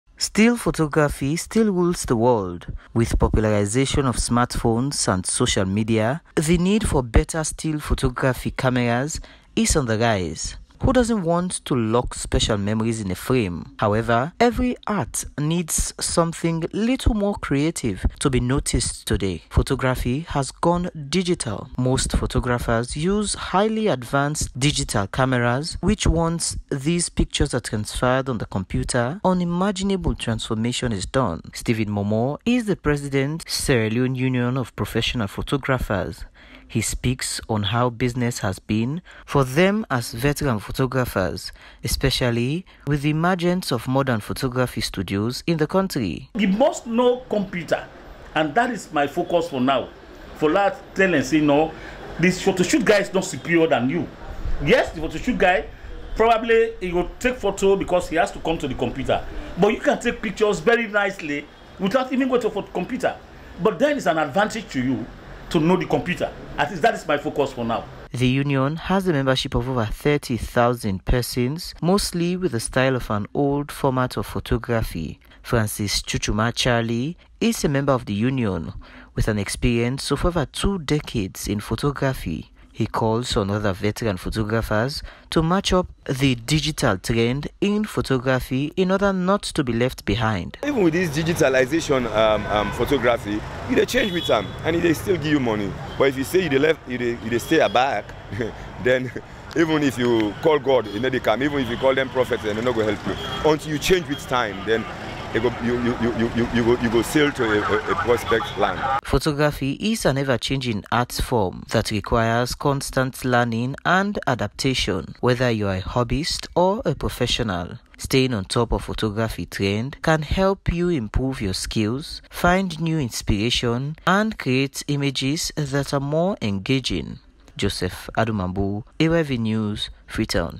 VETERAN PHOTOGRAPHERS IN SIERRA LEONE SEE NEED TOWARDS MODERN PHOTOGRAPHY
The President for Sierra Leone Union of Professional Photographers STEVEN ELOJIAMA MOMOH has encouraged all veteran photographers in the country to move with the global trend of modern photography by learning the computer; stating that even though they have a wider knowledge in photography, being vast and knowledgeable in computer programming will serve as an added advantage. He said this while speaking with our reporter JOSEPH ADU MAMBU on how business is, now that there is the emergence of modern photography studios in the country. Here is the story